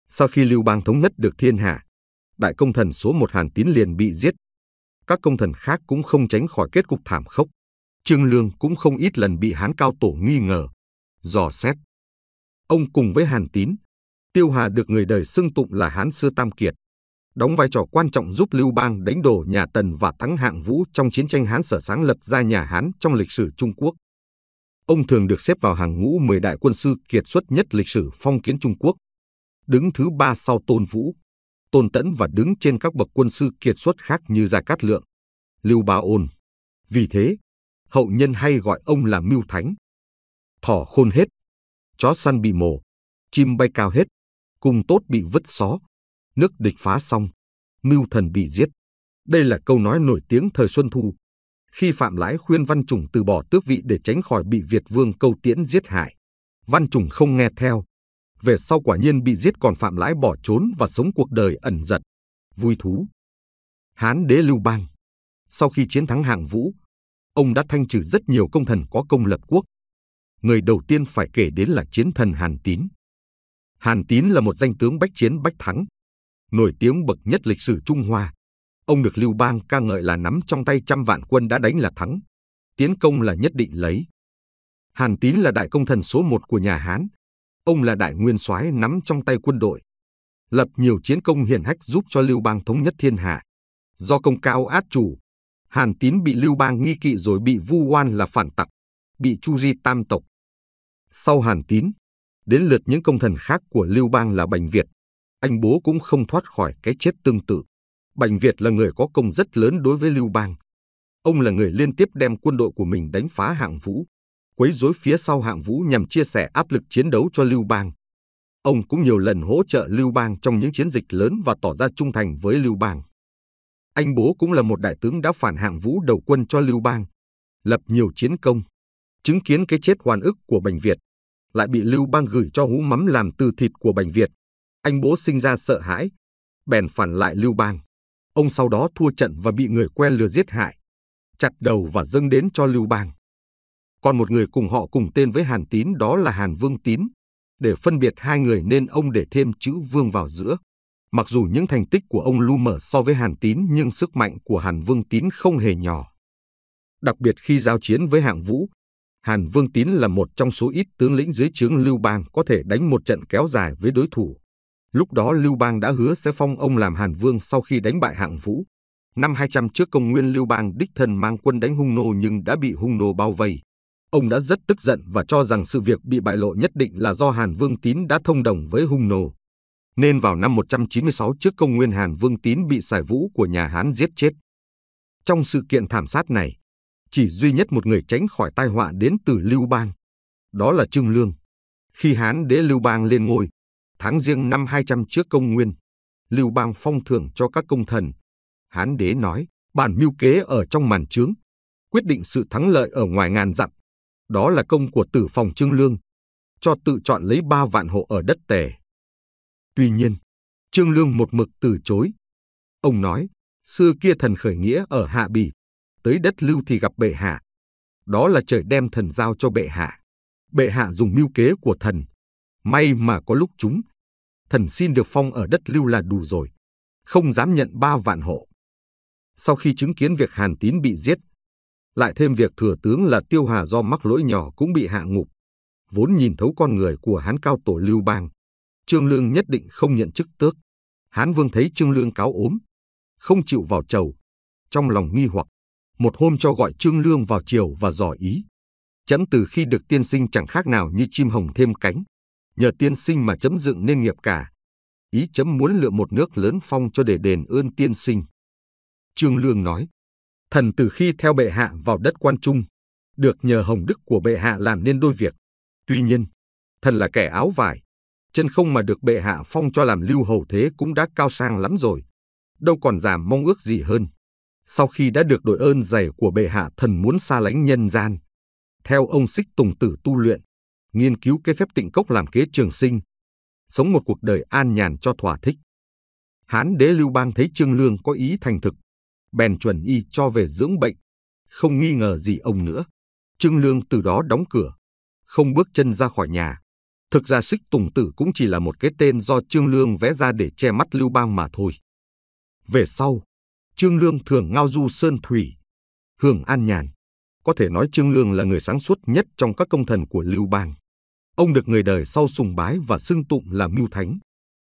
Lưu Bang thanh trừng công thần, Trương Lương học theo Phạm Lãi dùng trí thoát đòn
Sau khi Lưu Bang thống nhất được thiên hạ, đại công thần số 1 Hàn Tín liền gì bị giết, các công thần khác cũng không tránh khỏi kết cục thảm khốc. Trương Lương cũng không ít lần bị Hán Cao Tổ nghi ngờ, dò xét#truongluong#luubang#hancaoto